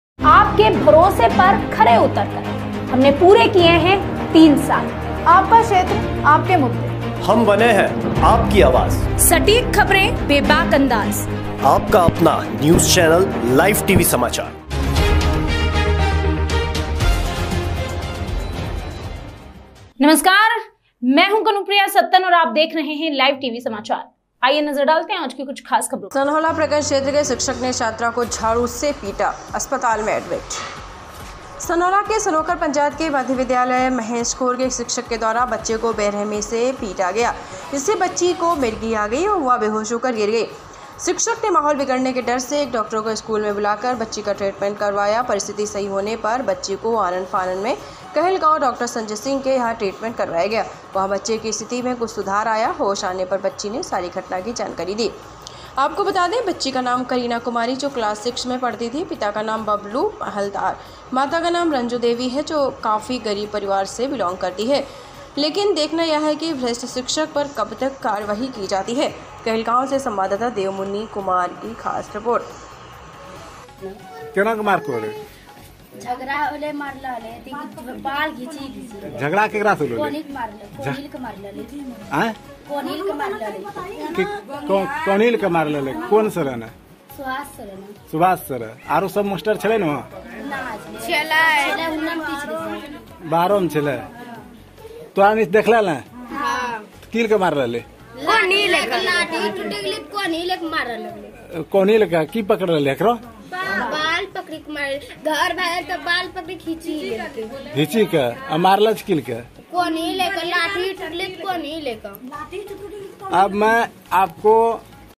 कहलगांव : सनहौला प्रखंड क्षेत्र के एक शिक्षक ने छात्रा को झाड़ू से पीटा अस्पताल में भर्ती
सनहौला प्रखंड क्षेत्र के एक शिक्षक ने छात्रा को झाड़ू से पीटा अस्पताल में भर्ती ।

सनहौला के सनोखर पंचायत के मध्य विद्यालय महेशखोर के एक शिक्षक के द्वारा बच्चे को बेरहमी से पीटा गया जिससे बच्ची को मिर्गी आ गया एवं वह वहीं पर बेहोश होकर गिर पड़ी शिक्षक ने माहौल बिगड़ने के डर से एक डॉक्टर को स्कूल में बुलाकर बच्ची का ट्रीटमेंट करवाया परिस्थिति सही नहीं होने पर बच्ची को आनन-फानन में कहलगांव डॉक्टर संजय सिंह के यहां  ट्रीटमेंट करवाया गया वहां बच्चे की स्थिति में कुछ सुधार आया होश आने पर बच्ची ने सारी घटना की जानकारी दी ।

मैं आपको बता दूं बच्ची का नाम करीना कुमारी जो क्लास सिक्स में पढ़ती थी पिता का नाम बबलू महाल्दार माता का नाम रंजो देवी है जो काफी गरीब परिवार से विलोम करती हैं लेकिन देखना यह है कि ऐसे भ्रष्ट शिक्षक पर कब तक कारवाई की जाती है

कहलगाँव से संवाददाता देवमुनी कुमार की रिर्पोट ।